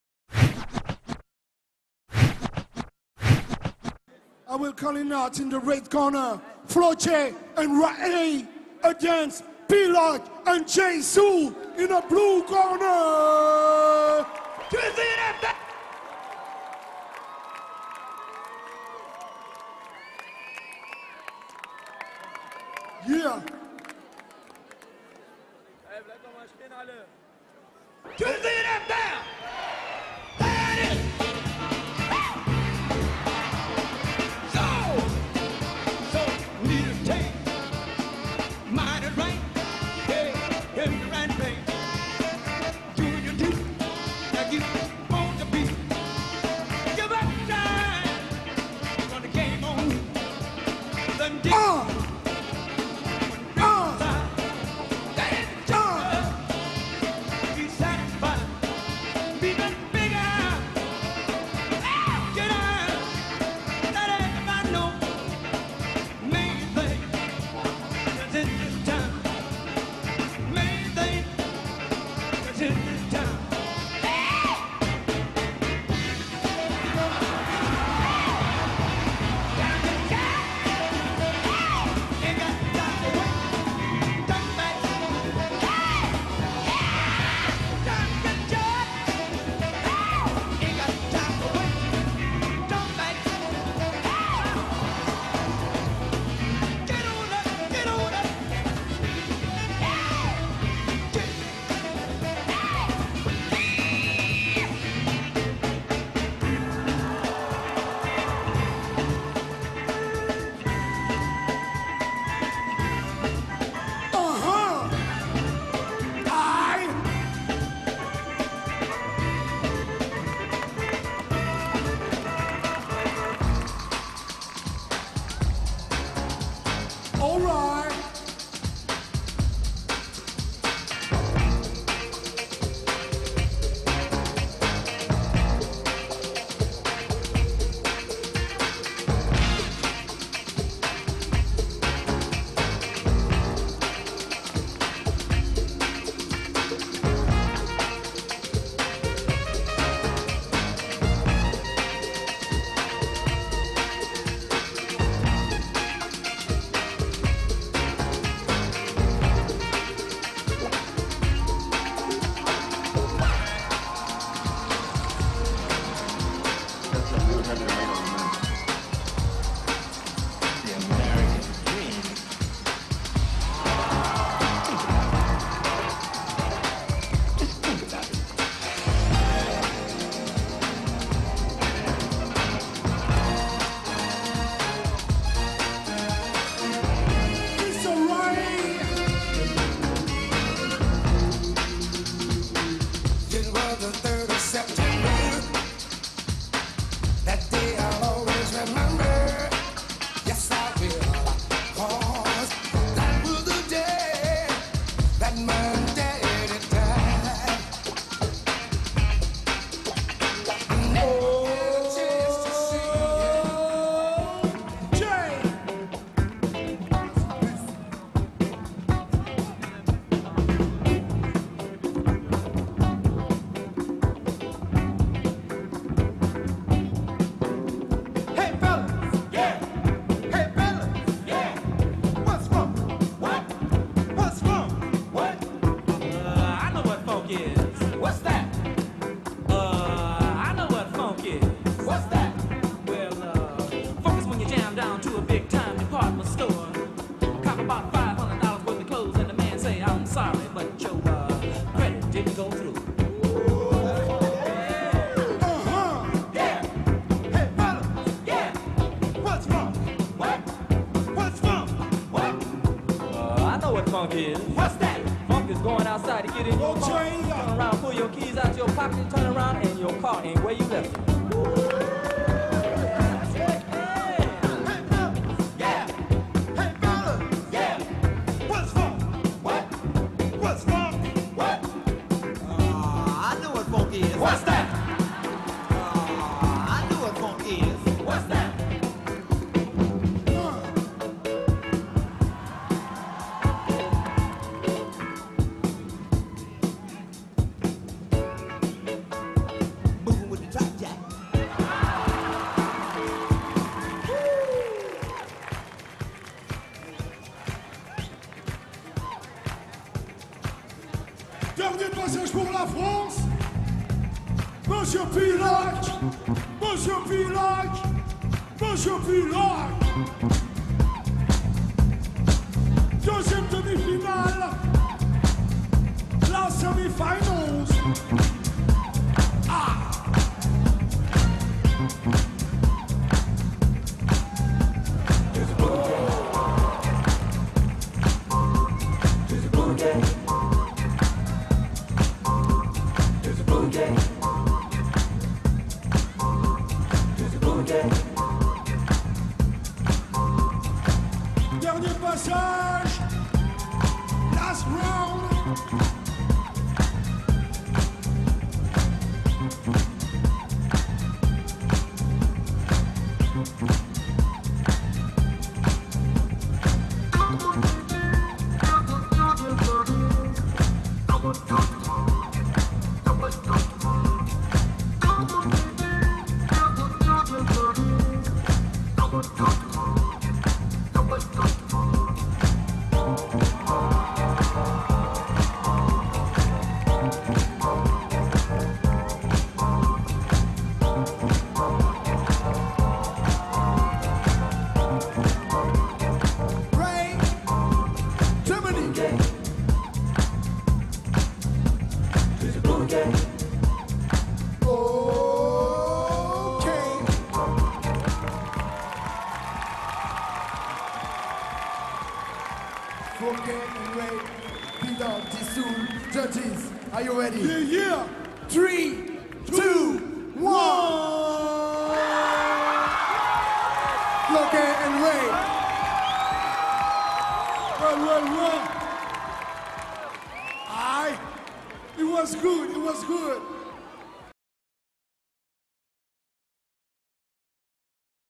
Locking Semi-Final - Juste Debout 2011 - P.Lock & J.Soul & Flockey & Rae
P.Lock & J.Soul (France)  VS.  Flockey & Rae (Germany) in semi final of locking in JUSTE DEBOUT 2011, the most important hip hop contest around the world.